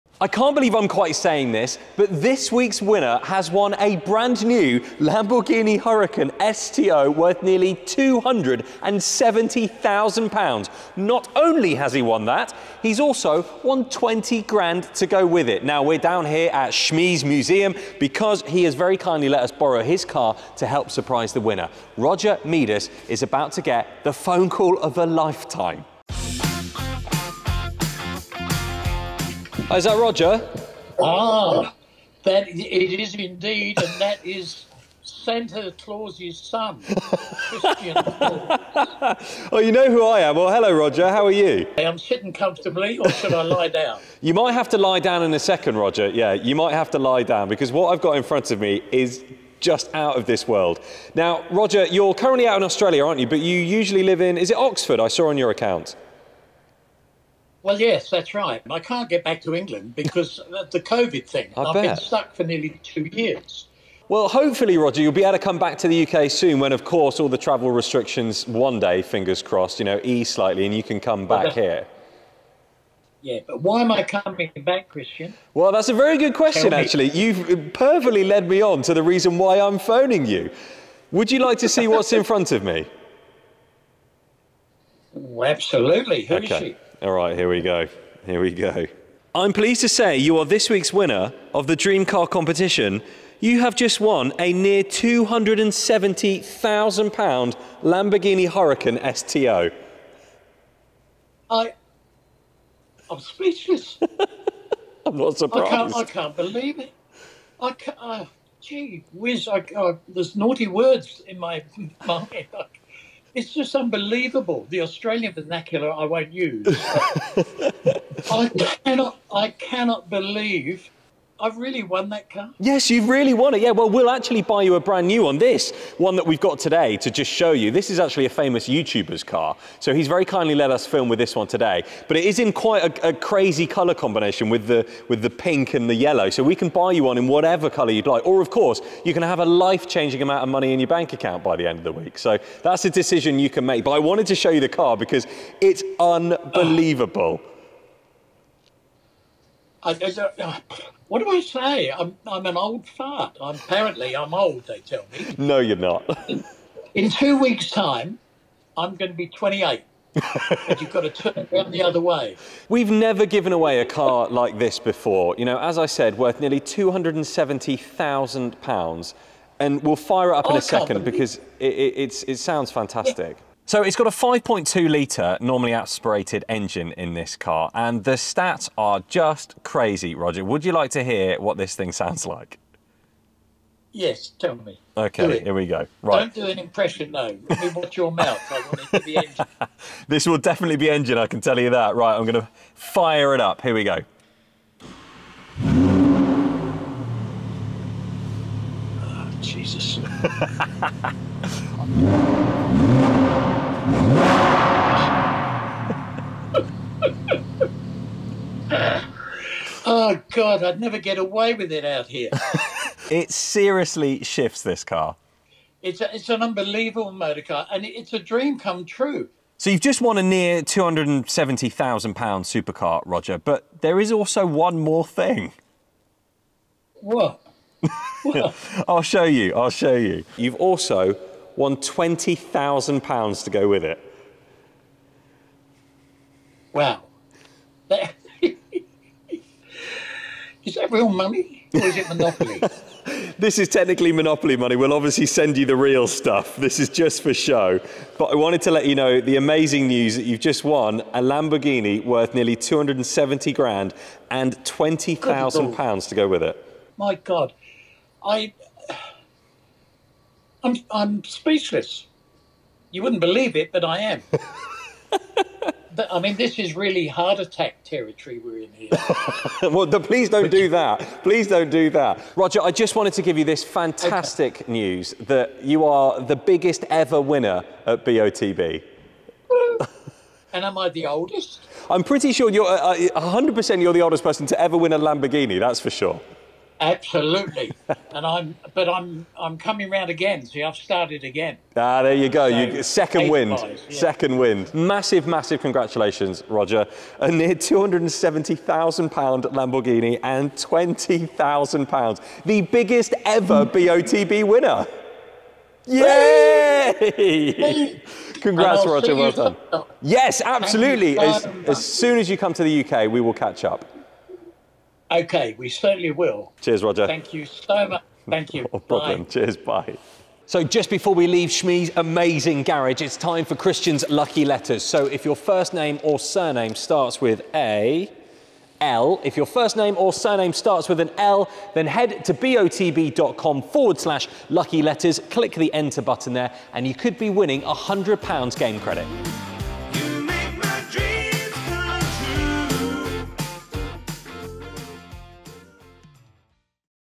82-Year-Old Wins £270,000 Lamborghini STO! | BOTB Biggest Ever Dream Car Winner
Win your Dream Car with BOTB | Enter the BOTB Dream Car, Midweek Car & Lifestyle Competitions from just 25p | Join our list of winners!

This week turned out to be a very special week, as we're giving away our biggest ever prize, the Lamborghini Huracan STO (worth almost £270,000!) to the unsuspecting Roger Medus!

A very smiley Roger was almost lost for words as Christian turned the camera round to reveal the bright pink STO! This 631bhp naturally aspirated V10 is ridiculously fast and seriously aggressive, not surprising considering it weighs a whole 100kg less than the Huracan Evo! What a car!

"G-whizz! I cannot believe I've really won that car!" Roger exclaimed. "This is a real dream come true!"

But the surprise wasn't over yet, as Christian went on to surprise Roger with a massive £20,000 cash – making this total prize worth almost £300,000!

Wow – what a win! Congratulations Roger!